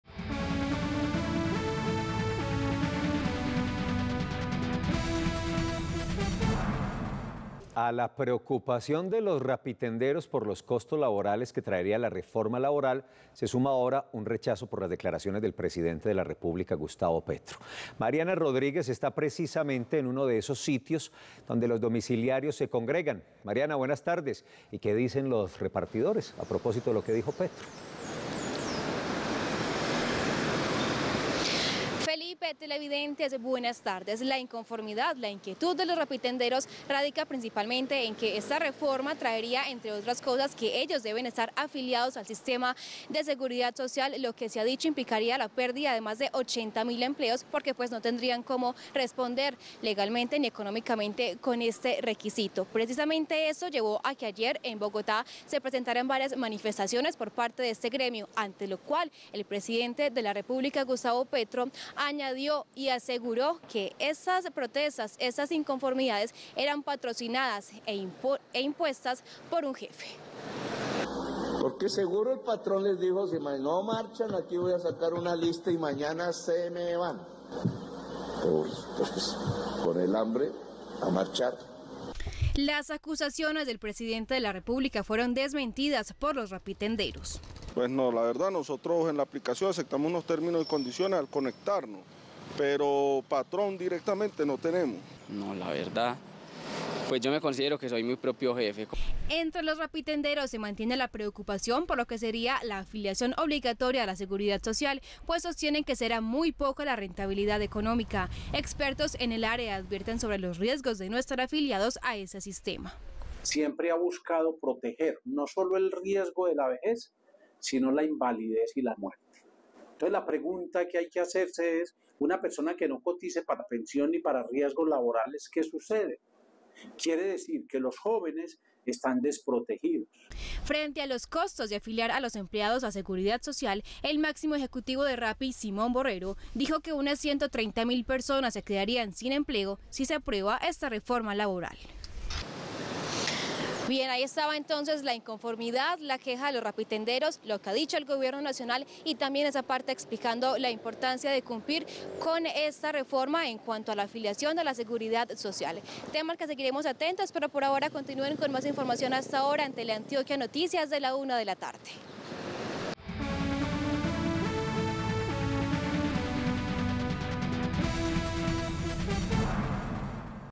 Rappitenderos dicen que no son manipulados - Teleantioquia Noticias
A la preocupación de los Rappitenderos por los costos laborales que traería la reforma laboral, se suma un rechazo por las declaraciones del presidente Gustavo Petro.